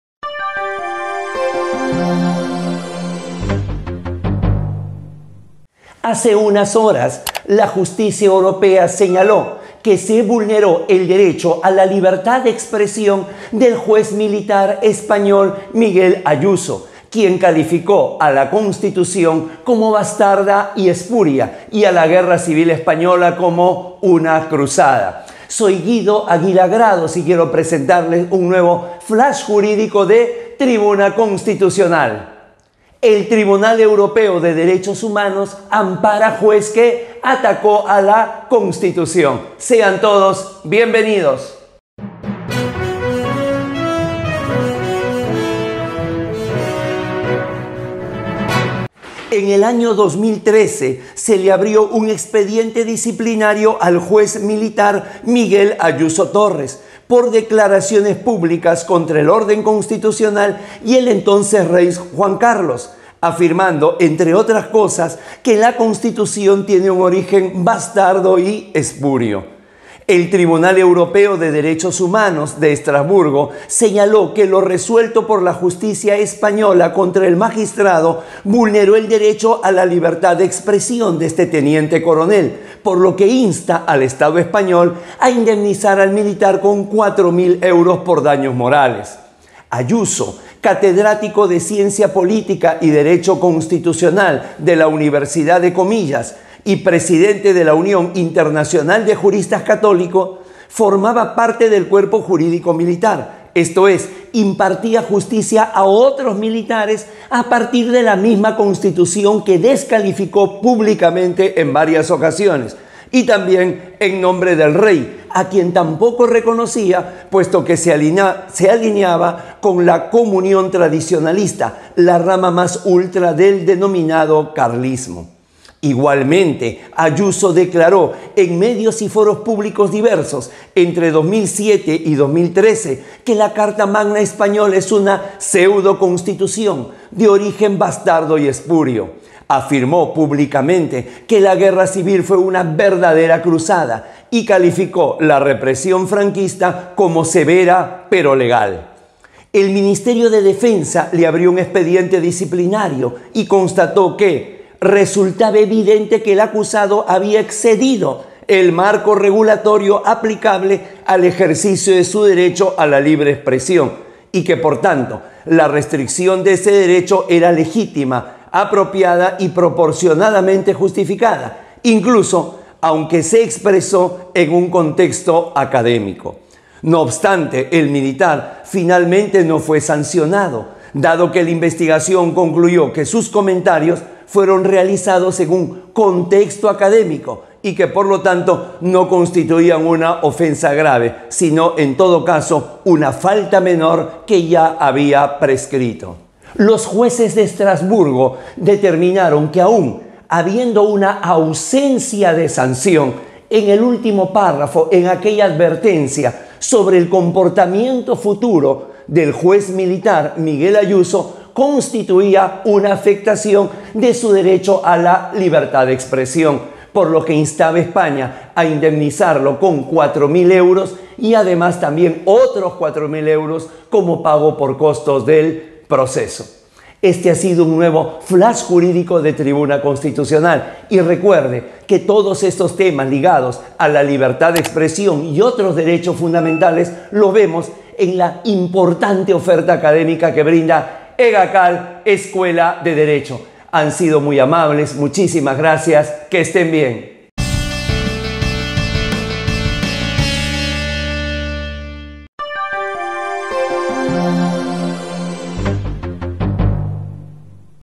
TEDH ampara a Juez que atacó la Constitución - FJ # 57
Hace unas horas el Tribunal Europeo de Derechos Humanos amparó al juez militar español Miguel Ayuso quien había calificado a la  Constitución de "bastarda y espúrea".
En la presente entrega desarrollamos la importante jurisprudencia sobre la libertad de expresión.

🔴𝐏𝐀𝐑𝐀 𝐂𝐎𝐍𝐒𝐔𝐋𝐓𝐀𝐒
📌⚖𝐂𝐮𝐫𝐬𝐨𝐬 𝐄𝐬𝐩𝐞𝐜𝐢𝐚𝐥𝐢𝐳𝐚𝐝𝐨𝐬 𝐲 𝐚𝐬𝐞𝐬𝐨𝐫í𝐚𝐬 𝐚𝐜𝐚𝐝é𝐦𝐢𝐜𝐚𝐬